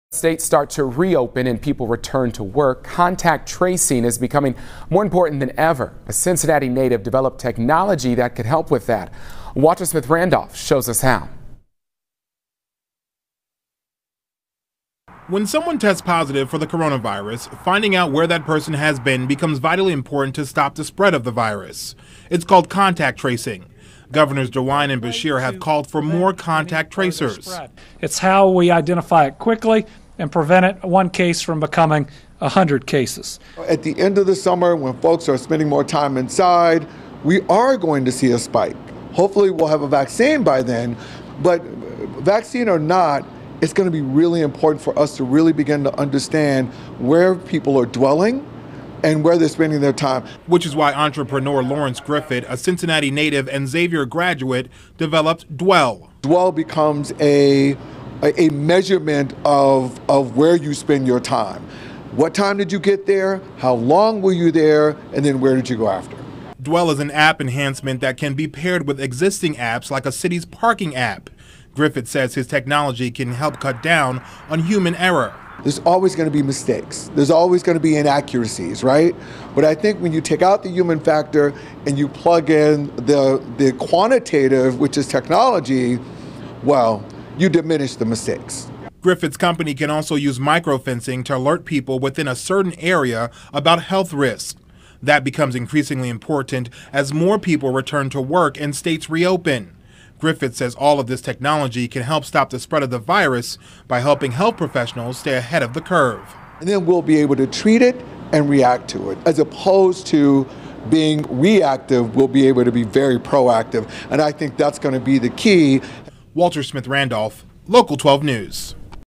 Digital Factory CEO Lawrence Griffith on WKRC Cincinnati - COVID-19 Contact Tracing.
Digital Factory CEO talks about the advantages of using patented DWELL technology to increase the accuracy of contact tracing.